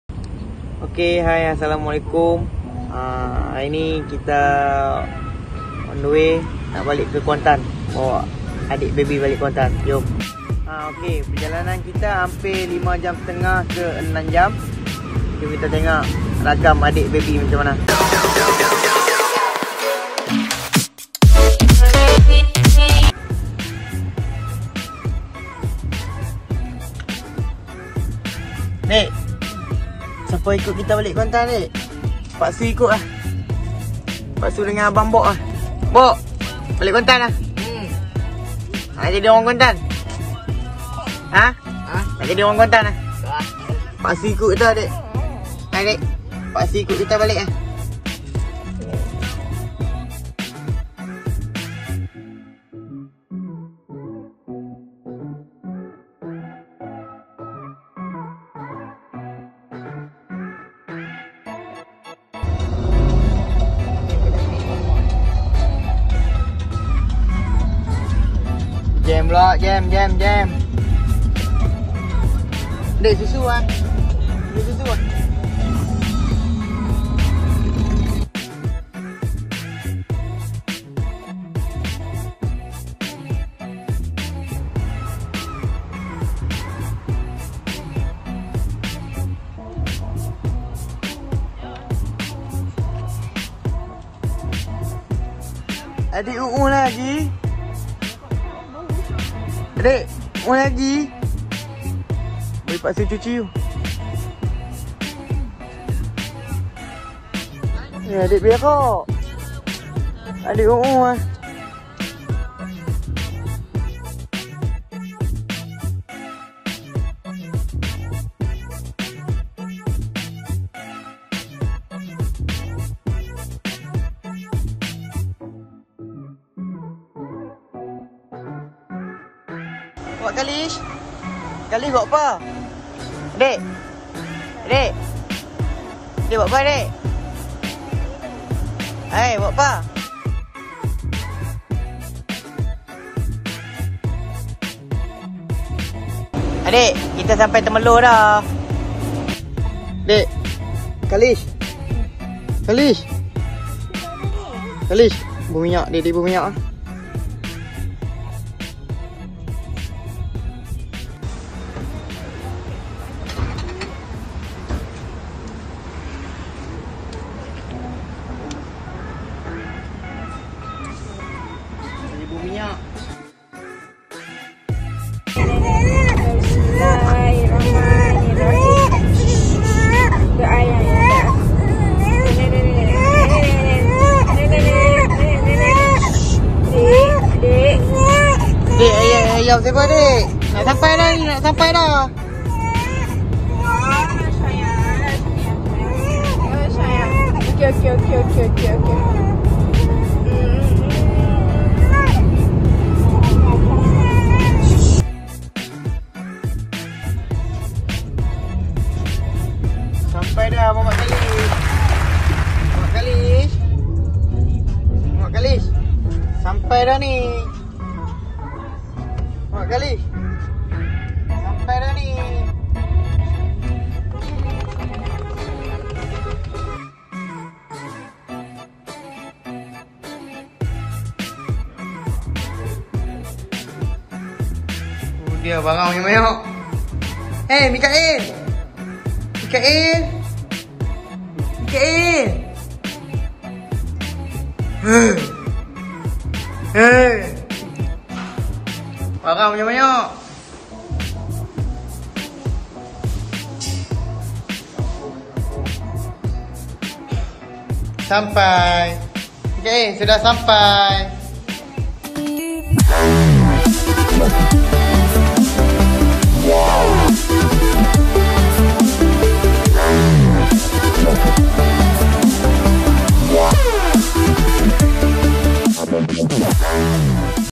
Baby Mickey Balik Kuantan
Setelah beberapa bulan adik baby berada di kampung Penang jadi hari ini Dady Mickey bawak balik Kuantan. Jom tonton ragam adik baby